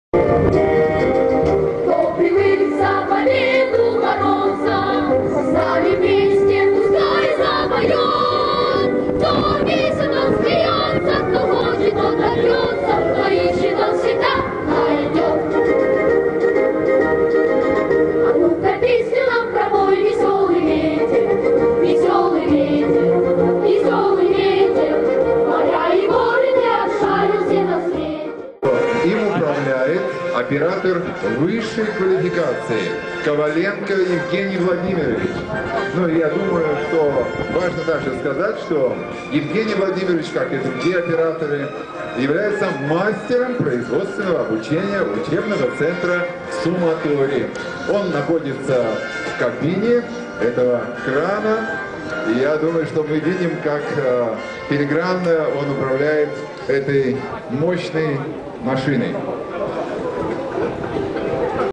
Рабочих кадров в Приморье станет больше
В Артеме открылся современный учебный центр по подготовке и переподготовке кадров